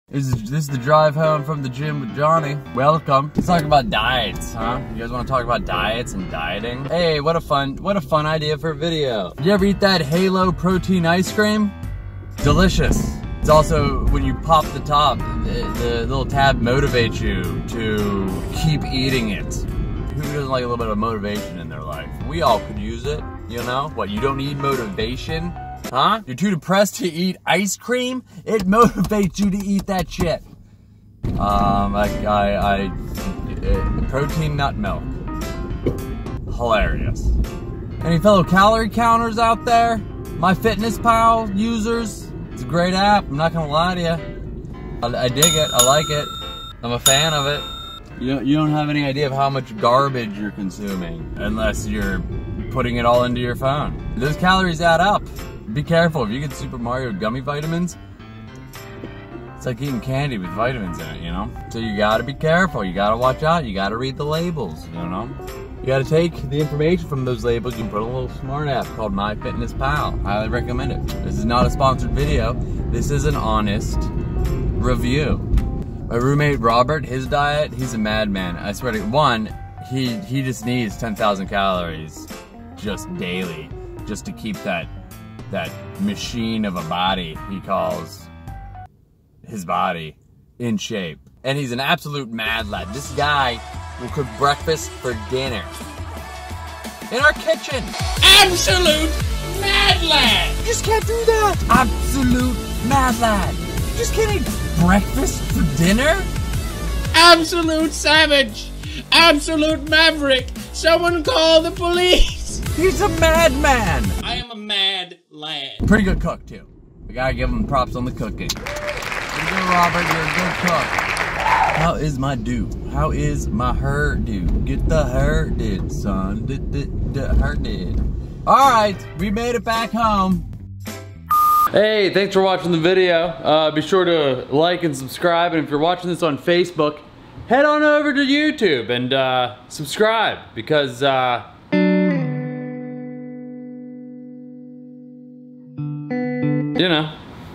Roommate Review & Diet Review - Gym Drive #1 (100% ORIGINAL CONTENT)
Ya wanna talk diets and roommates? Of course you do. Special thanks to Pewds for the ORIGINAL CONTENT. What an Absolute Mad Lad.

Robots on Fire - Johnny Costabile & Robert Ernst be the duders in the vid.